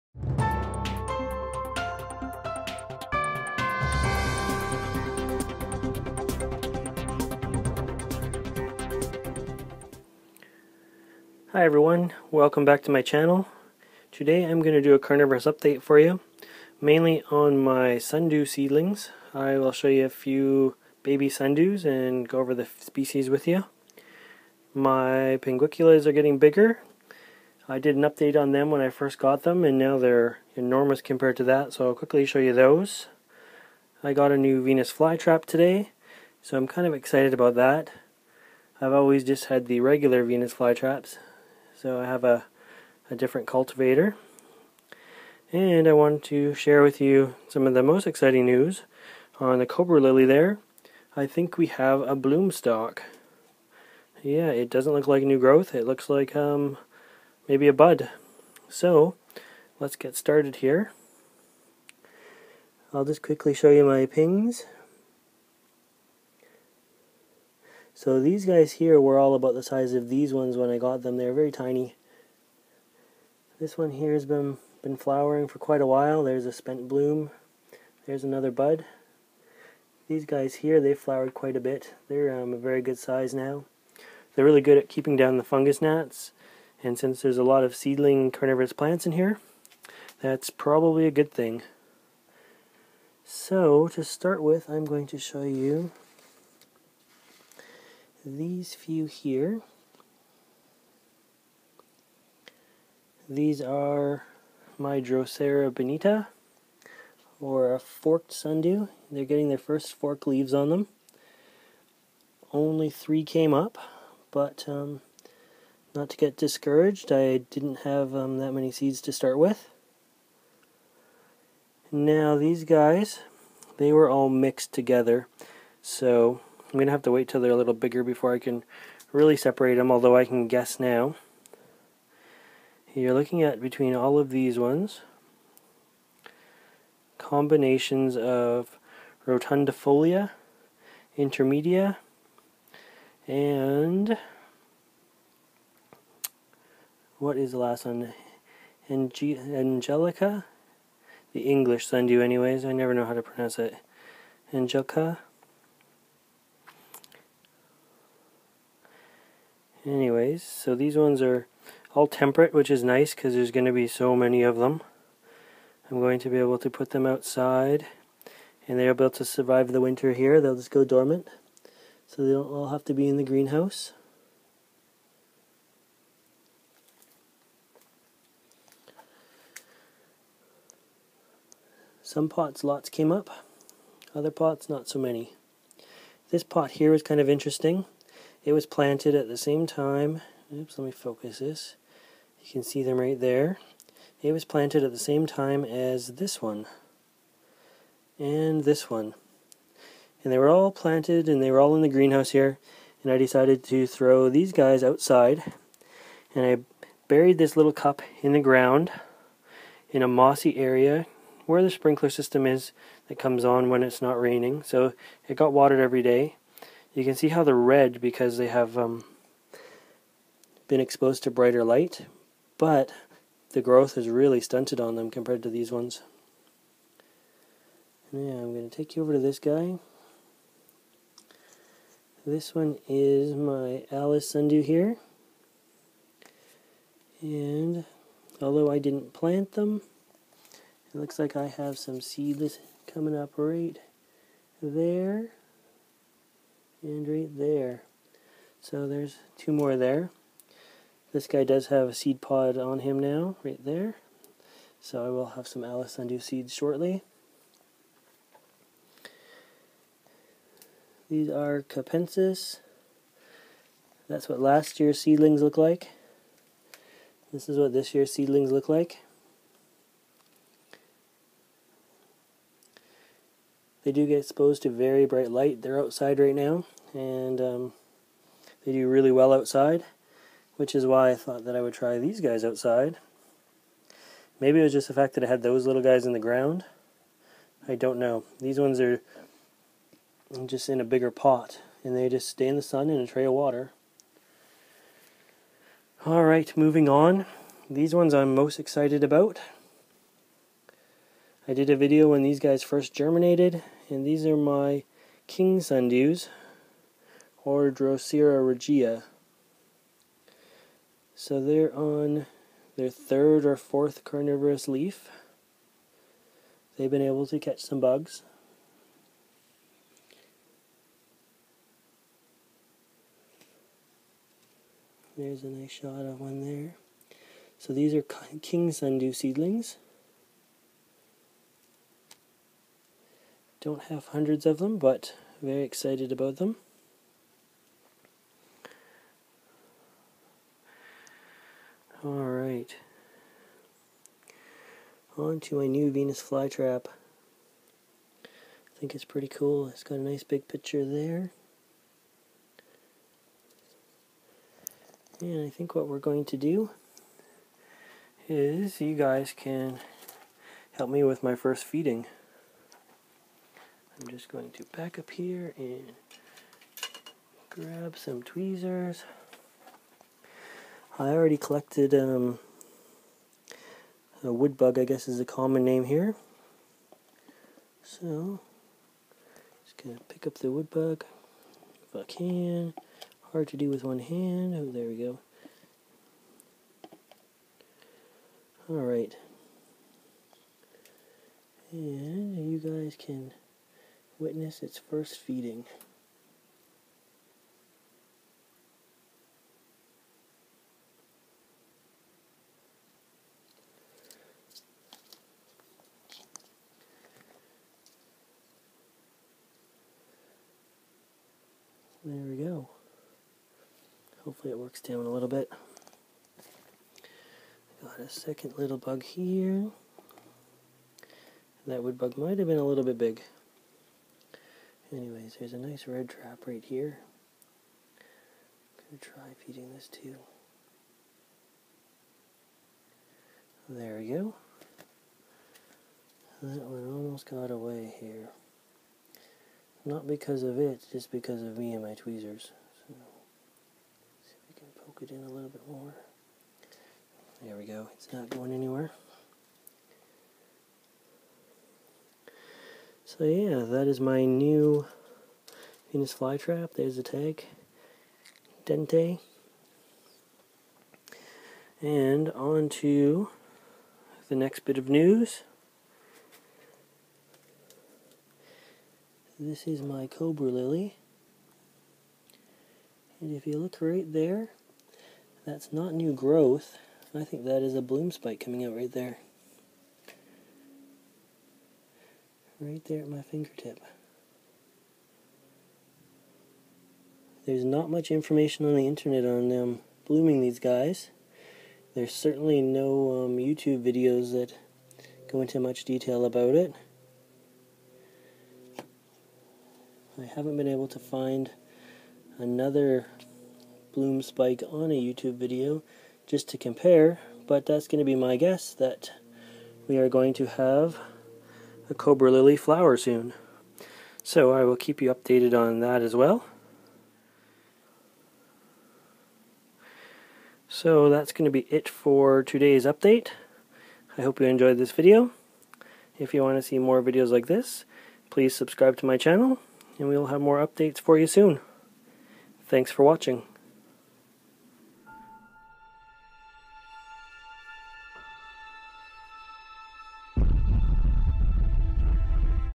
Carnivorous Plant Update: King sundew seedlings, Cobra lily bloom spike & feeding new Venus Fly trap
June Carnivorous Plant June Update: King sundew seedlings and so many other Drosera seedlings, Cobra lily bloom spike, huge Ping/ Butterwort growth & feeding my new Venus Fly trap.